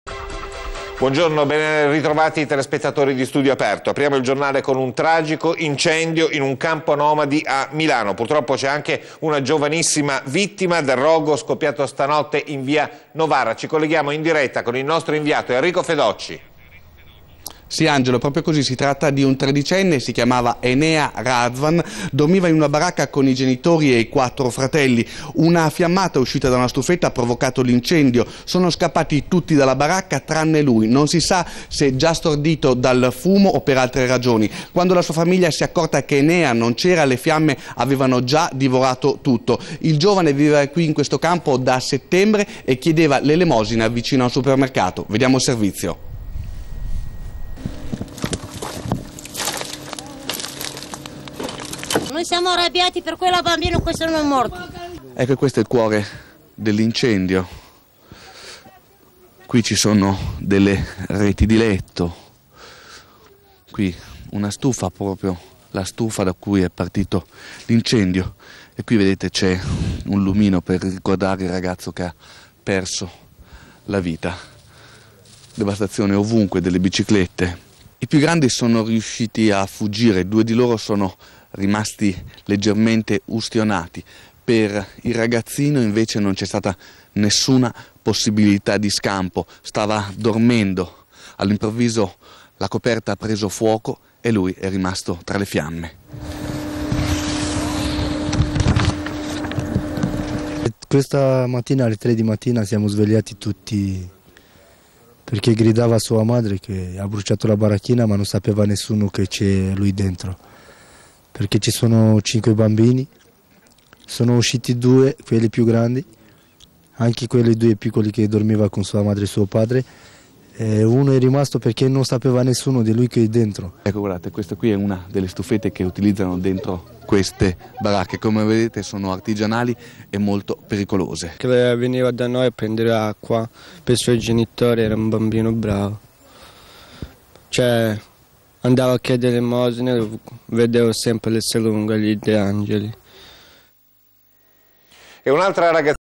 Rom 13enne muore bruciato a Milano
Un ragazzo di 13 anni è morto nell'incendio della propria baracca in un campo nomadi a Milano. Secondo quanto riferito dal padre della vittima, il giovane stava alimentando una stufa a legna quando sarebbe partita una fiammata che ha incendiato la baracca. I familiari hanno tentato di spegnere le fiamme ma queste hanno in poco tempo attaccato tutta la struttura. Il padre ha portato tutti in salvo tranne il ragazzo che è deceduto fra le fiamme.